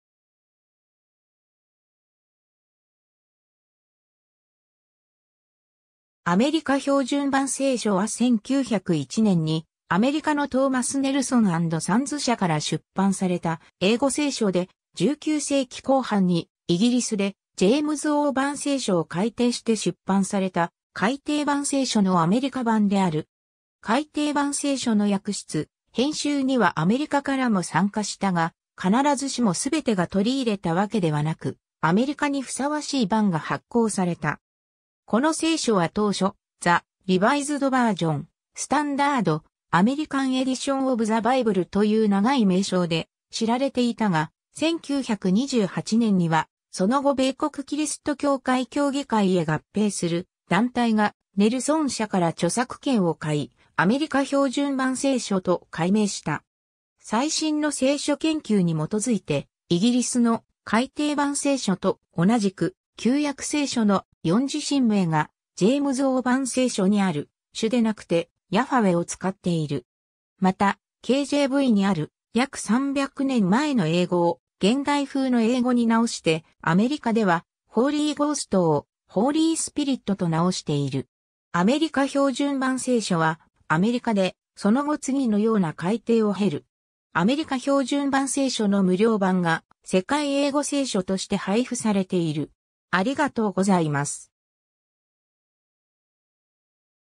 アメリカ標準版聖書
『アメリカ標準版聖書』（アメリカひょうじゅんばんせいしょ、英語: American Standard Version、略称：ASV）は1901年にアメリカのトーマス・ネルソン＆サンズ社（Thomas Nelson & Sons）から出版された英語聖書で、19世紀後半にイギリスでジェームズ王版聖書（1611年出版）を改訂して出版された『改訂版聖書』のアメリカ版である。
 『改訂版聖書』の訳出・編集にはアメリカからも参加したが、必ずしもすべてが取り入れたわけではなく、アメリカにふさわしい版が発行された。
 この聖書は当初『The Revised Version, Standard American Edition of the Bible』という長い名称で知られていたが、1928年にはその後米国キリスト教会協議会へ合併する団体がネルソン社から著作権を買い、『アメリカ標準版聖書』と改名した。
 最新の聖書研究に基づいて、イギリスの『改訂版聖書』と同じく、旧約聖書の「四字神名」（YHVH）が『ジェームズ王版聖書』（KJV）にある「主」（the Lord）でなくて、「ヤハウェ」（Jehovah）を使っている。
また、KJVにある約300年前の英語を現代風の英語に直して、アメリカではHoly Ghost（聖霊）をHoly Spiritと直している。
 『アメリカ標準版聖書』はアメリカで、その後次のような改訂を経る。
 『アメリカ標準版聖書』の無料版が『世界英語聖書』（World English Bible、1997年、略称：WEB）として配布されている。